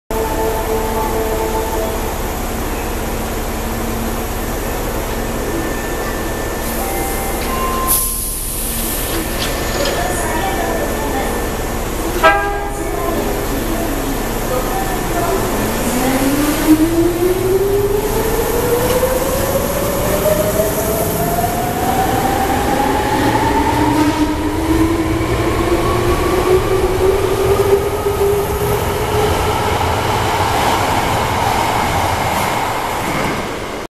阪急3300系 堺筋線南森町駅発車 Hankyu 3300 series EMU
阪急千里線～大阪市営地下鉄堺筋線直通天下茶屋行き電車
Hankyu Railways commuter train at Osaka subway Sakaisuji line.